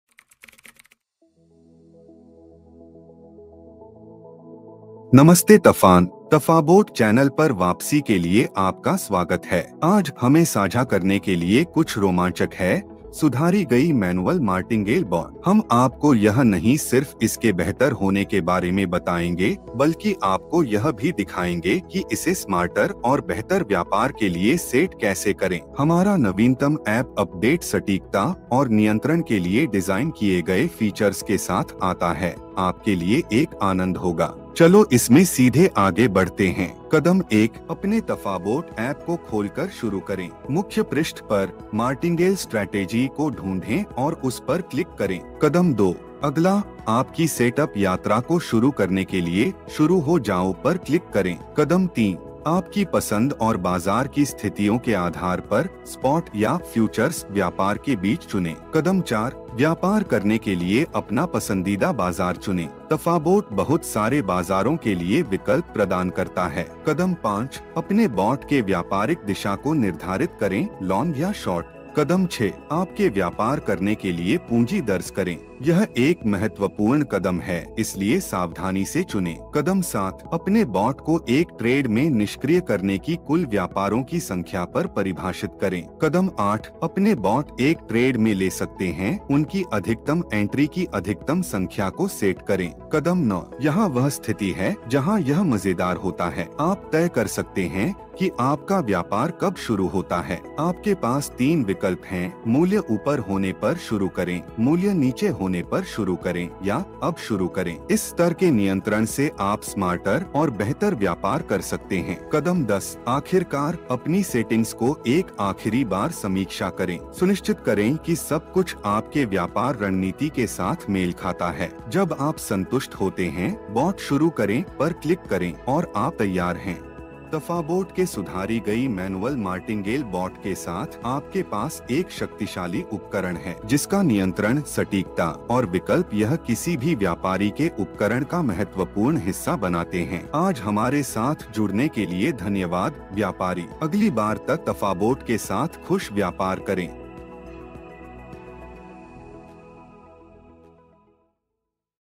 Tafabot का मैन्युअल मार्टिंगेल बॉट - कदम से कदम ट्यूटोरियल
Tafabot के आधिकृत चैनल पर आपका स्वागत है! इस वीडियो में, हम आपको हमारे एन्हांस्ड मैन्युअल मार्टिंगेल बॉट का परिचय देंगे - आपके स्मार्टर ट्रेडिंग के लिए एक प्रवेश द्वार।

हम आपको इसे सेटअप करने के माध्यम से लेकर जाएंगे और आपको तीन शानदार विकल्पों के साथ आपके व्यापारों के लिए आदर्श समय का चयन कैसे करना है वो दिखाएंगे।

चाहे आप पुराने व्यापारी हों या अभी शुरू कर रहे हों, यह उपके व्यापार के उपकरण के रूप में महत्वपूर्ण है।

Tafabot के साथ खुश व्यापार करें!